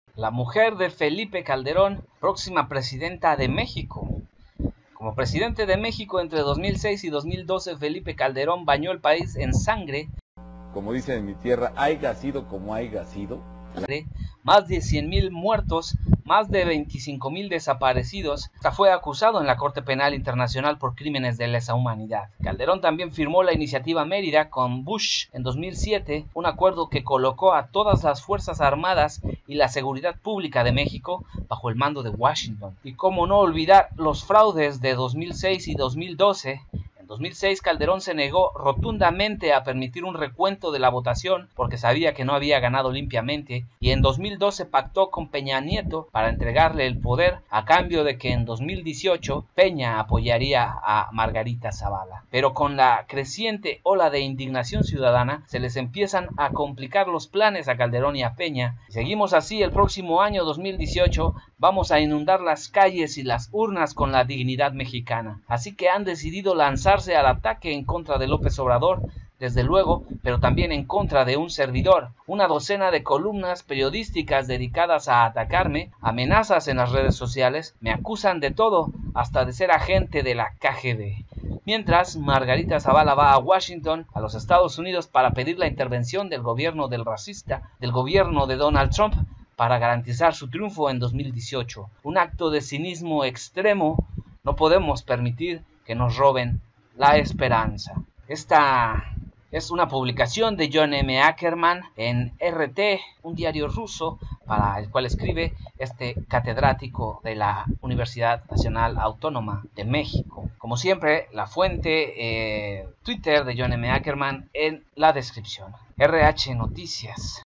¿La mujer de Felipe Calderón, próxima presidenta de México?
Me acusan de todo, hasta ser agente de la KGB
Fuente: RT